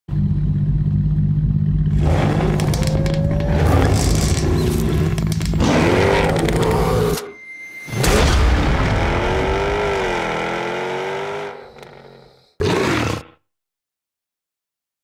The Attitude Sport Award 2018, supported by Jaguar
Be Your Own Animal. The Attitude Sport Award, supported by Jaguar at the Virgin Holidays Attitude Awards 2018, powered bu Jaguar.